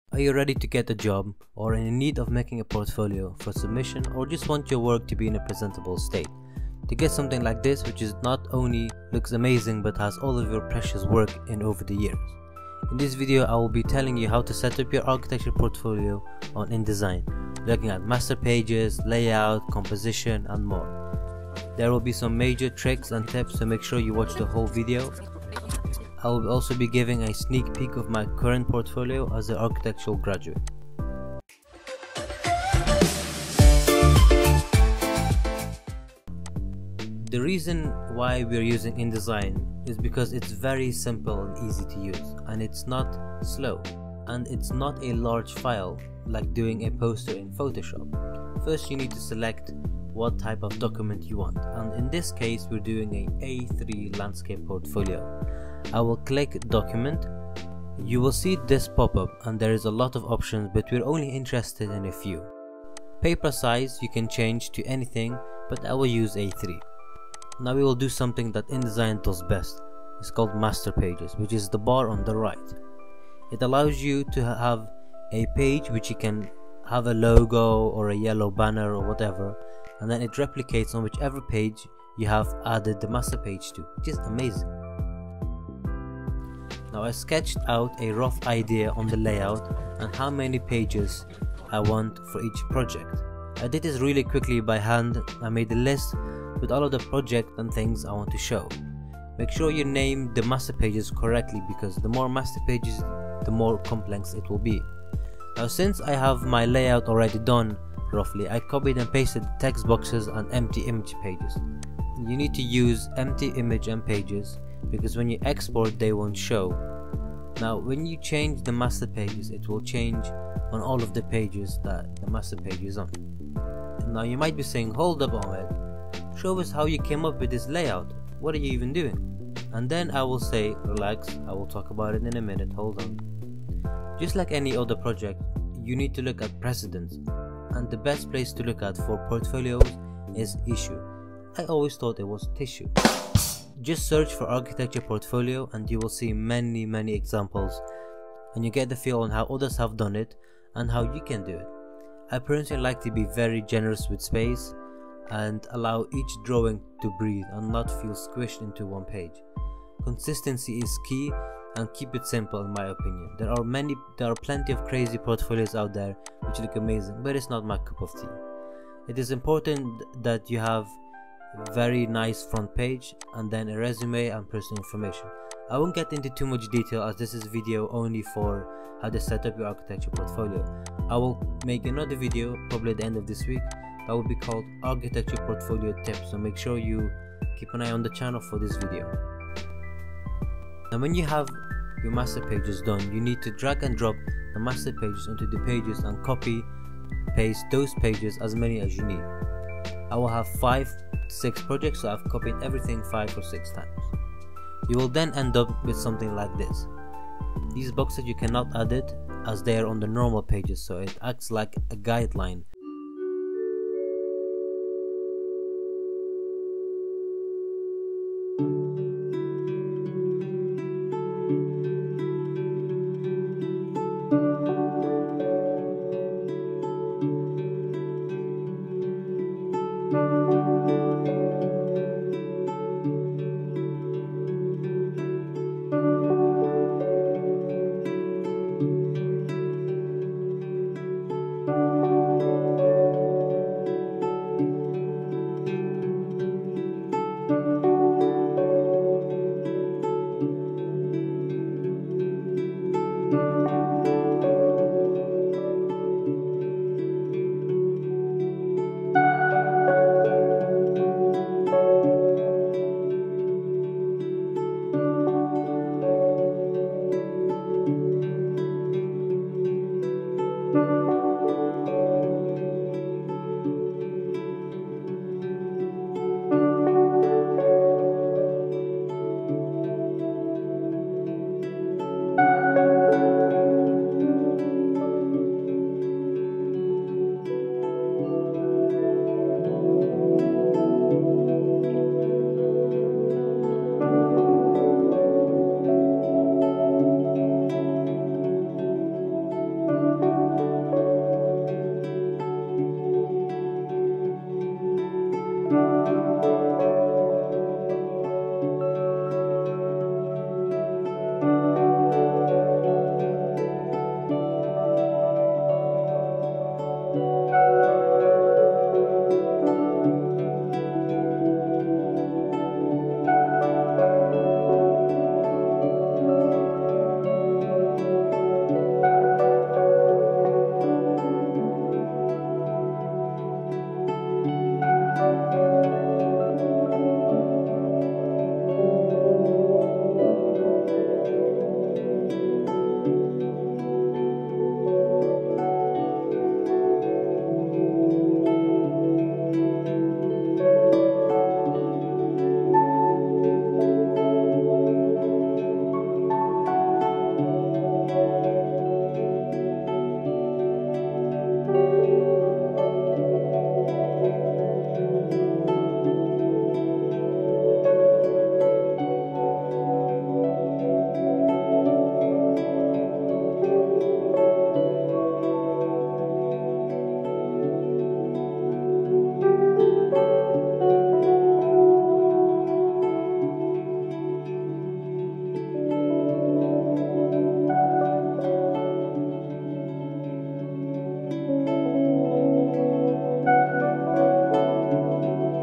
How to make a portfolio for Architects, Interns and Students in Adobe InDesign
InDesign Tutorial How to Design an Architecture Portfolio

In today's video, I will be teaching you guys how to use InDesign to create a portfolio for you as an undergraduate to start working. I have gone through Indesign tutorials before so make sure to check out that video on my channel. I have also shown you how to build a design portfolio. I have used Adobe InDesign which is a very useful tool to create portfolios.

- Even if you want me to look at your work pls do send me emails.